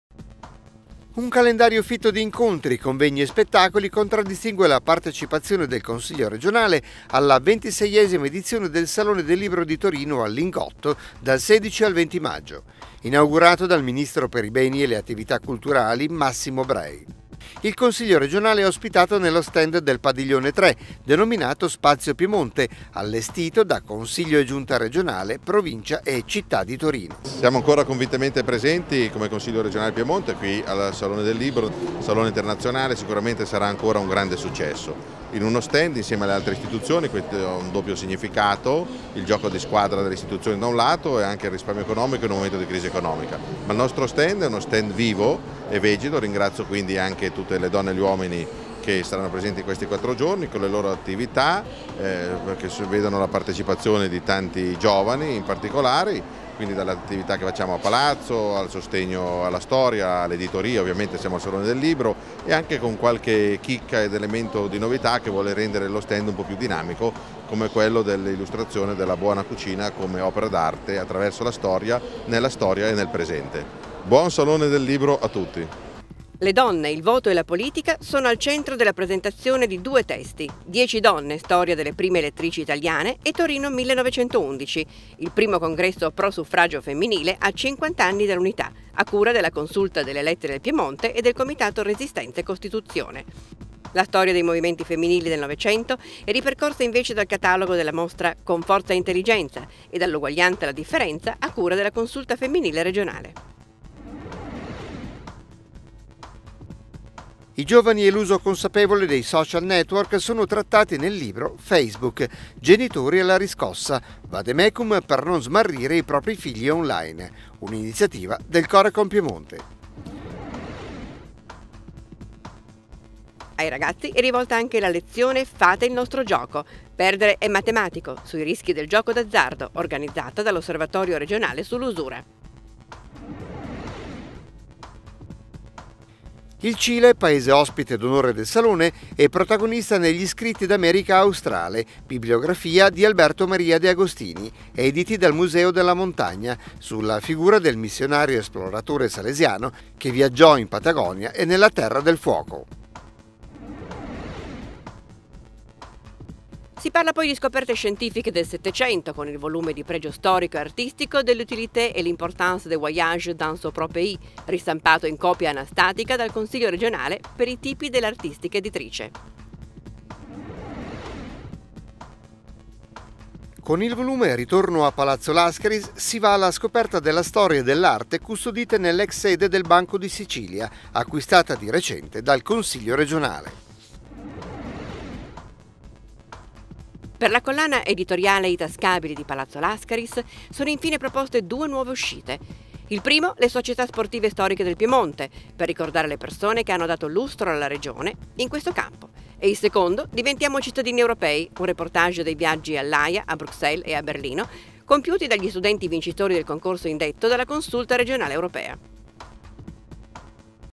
Il Consiglio regionale al Salone del Libro
Un calendario fitto di incontri, convegni e spettacoli contraddistingue la partecipazione del Consiglio regionale alla 26esima edizione del Salone del Libro di Torino al Lingotto dal 16 al 20 maggio. Inaugurato dal ministro per i Beni e le Attività Culturali, Massimo Bray. Il Consiglio regionale è ospitato nello stand del padiglione 3, denominato Spazio Piemonte, allestito da Consiglio e Giunta regionale, Provincia e Città di Torino.
[intervista al presidente Valerio Cattano]

* Le donne, il voto e la politica sono al centro della presentazione di due testi Dieci donne. Storia delle prime elettrici italiane e Torino 1911. Il primo congresso pro suffragio femminile a cinquant'anni dall'unità, a cura della Consulta delle Elette del Piemonte e del Comitato Resistenza e Costituzione. La storia dei movimenti femminili nel '900 è ripercorsa invece dal catalogo della mostra Con forza e intelligenza e Dall'uguaglianza alla differenza, a cura della Consulta femminile regionale.

* I giovani e l'uso consapevole dei social network sono trattati nel libro Facebook: genitori alla riscossa. Vademecum per non smarrire i propri figli online, un'iniziativa del Corecom Piemonte.

* Ai ragazzi è rivolta anche la lezione Fate il nostro gioco. Perdere è matematico, sui rischi del gioco d'azzardo, organizzata dall'Osservatorio regionale sull'usura.

* Il Cile, paese ospite d'onore del Salone, è protagonista negli Scritti d'America australe: bibliografia di Alberto Maria De Agostini, editi dal Museo della montagna, sulla figura del missionario esploratore salesiano che viaggiò in Patagonia e nella Terra del fuoco.

* Si parla poi di scoperte scientifiche nel '700 con il volume di pregio storico e artistico De l'utilité et de l'importance des voyages dans son propre pays, ristampato in copia anastatica dal Consiglio regionale per i tipi de L'Artistica editrice.

* Con il volume Ritorno a Palazzo Lascaris si va alla scoperta della storia e dell'arte custodite nell'ex sede del Banco di Sicilia, acquistata di recente dal Consiglio regionale.

* Per la collana editoriale I tascabili di Palazzo Lascaris sono infine proposte due nuove uscite. Il primo, Le società sportive storiche del Piemonte, per ricordare le persone che hanno dato lustro alla regione in questo campo e il secondo, Diventiamo cittadini europei, un reportage dei viaggi all'Aja, a Bruxelles e a Berlino compiuti dagli studenti vincitori del concorso indetto dalla Consulta regionale europea.